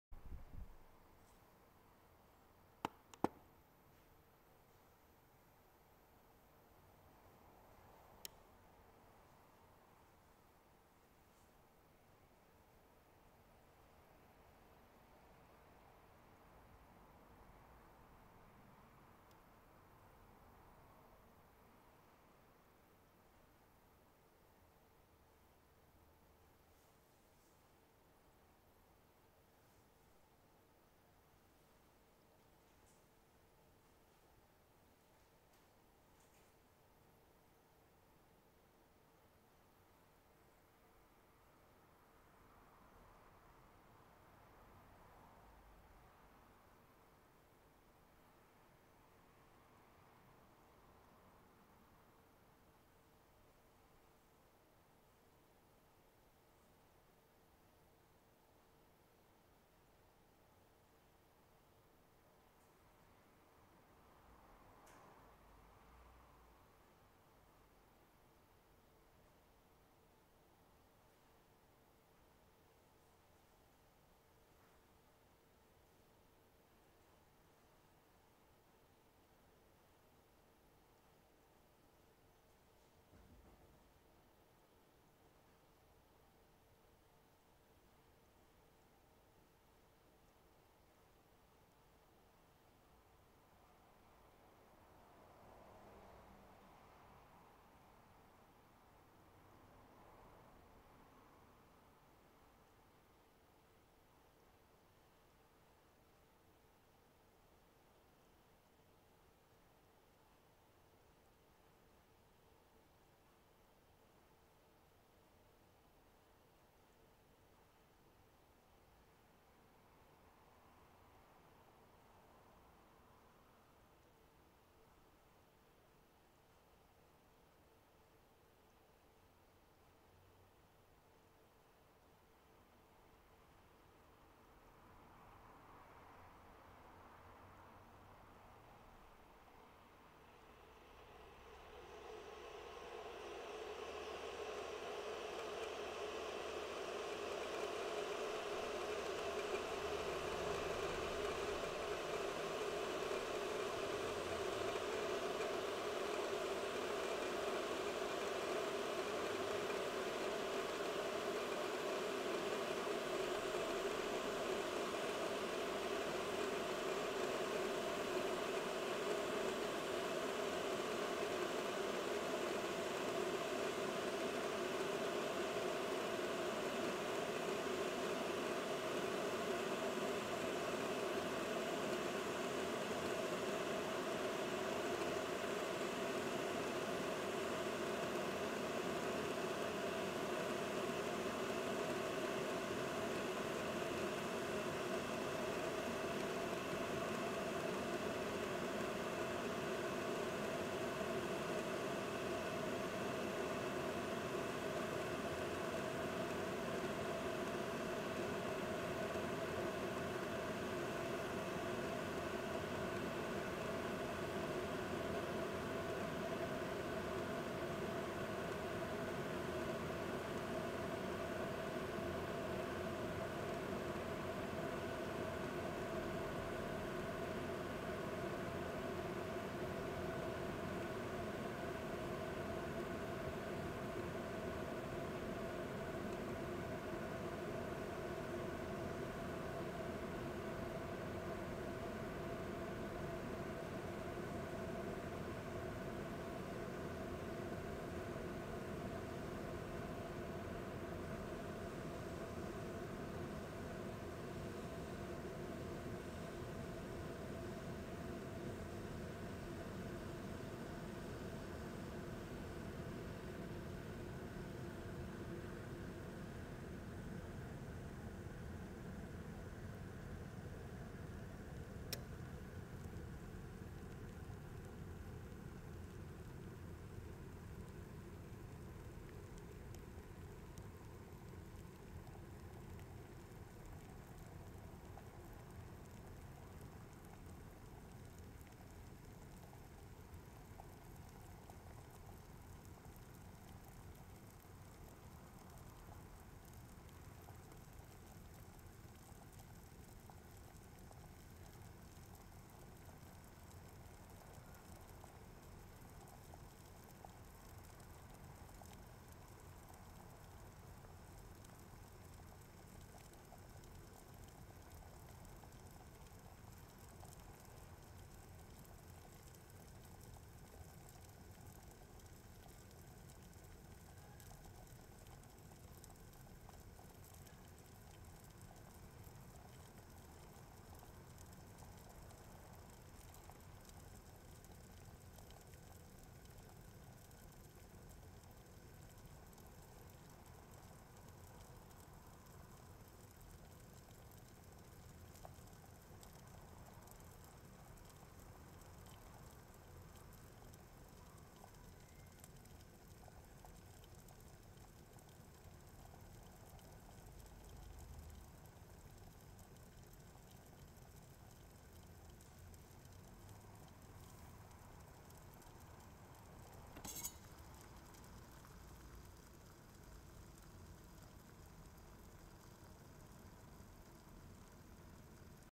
COFFEE ASMR Fellow Stagg EKG Kettle SLEEP INDUCING
Sleep-inducing coffee ASMR with Fellow's Stagg EKG Kettle.